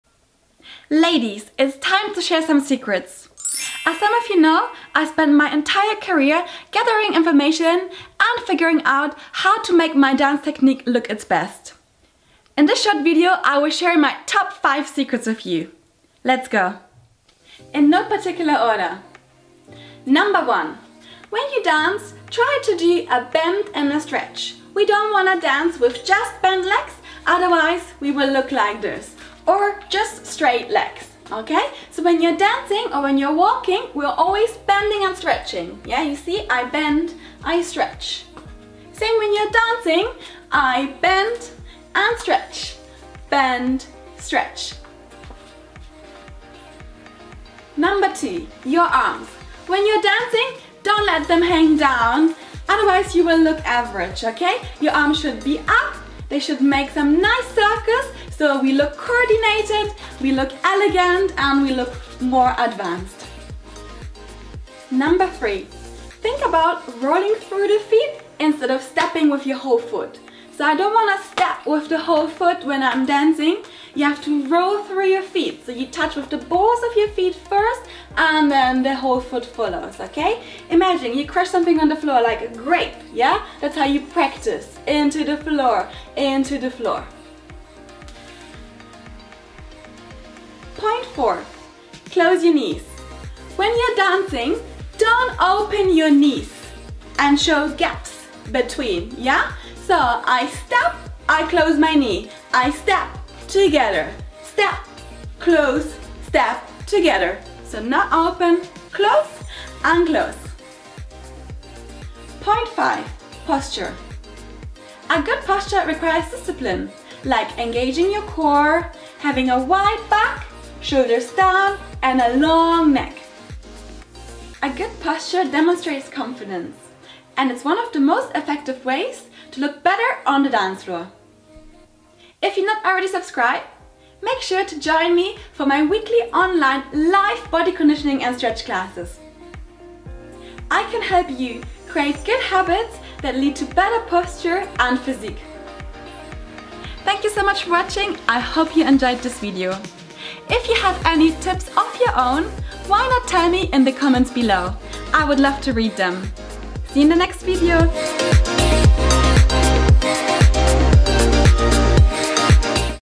5 TOP TIPS every BACHATA dancer should know - Ladies Styling by Melitta - DANCE SECRETS REVEALED !!!
The Ultimate DANCE TUTORIAL created for YOU!
TODAY'S TOPIC: My TOP 5 SECRETS for Bachata LADY STYLE by Melitta Siomos

If you are wondering why the professionals look so good on the dance floor you need to watch this video. Here I break down my top 5 tips to help you master your Bachata techniques.

Practice these techniques to perfect your Bachata movements. Let me know if you have any secret techniques of your own?

Learn Online with me and practice all the tips & tricks. FOR ALL LEVELS!

Did you like the video? Was it easy to follow? ❤️️
Let me know what other types of tutorials you would like me to do?

Bachata Lady Style | Estilo de chicas